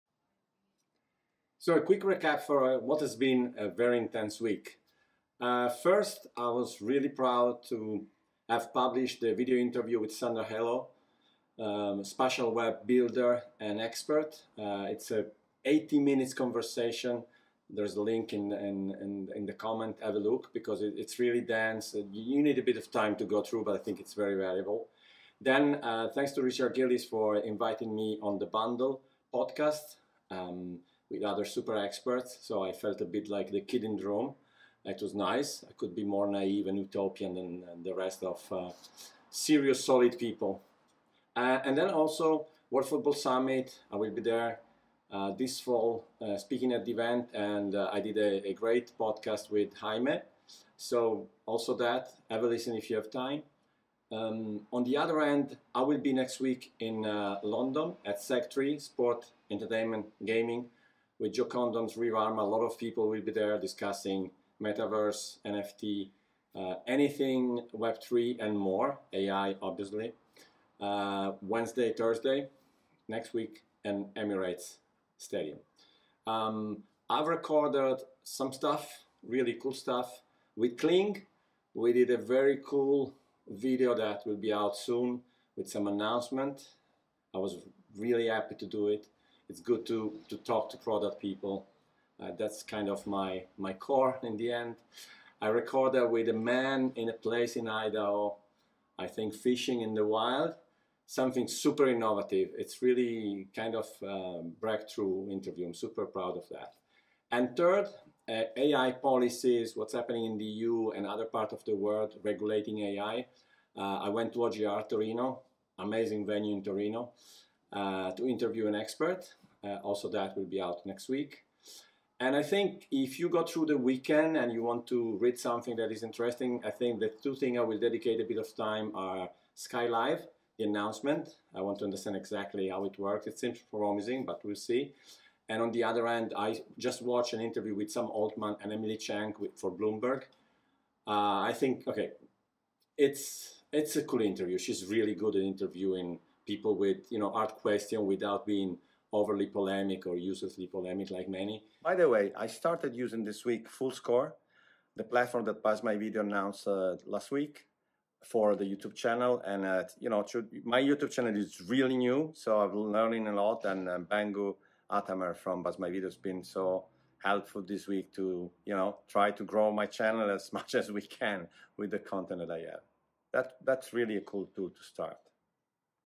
Spatial web, The Bundle podcast, WFS podcast, SEG3 London, Fullscore, YouTube, SKY Live
Recap of the week:
1. Super proud of the Spatial web and more with Sandra Helou video and article, she's a no-nonsense superstar
2. Loved to be involved in the latest episode of The Bundle podcast by Richard Gillis
3. Another day another podcast with Jaime Domínguez Pérez de Ayala and World Football Summit
4. I will be in London Wednesday to Friday next week for SEG3 with Joe Condon and Sree Varma and many others. Let's meet.
6. I have recorded a breakthrough innovation video with a man in Idaho, will be out on Tuesday
7. I have recorded a lady AI policy expert interview at OGR Torino, will be out next week
8. I have started using BuzzMyVideos Fullscore to grow my YouTube channel
Have a great weekend you all.

Suggested for the weekend:
1. SKY Live announcement
2. Sam Altman and Emily Chang interview for Bloomberg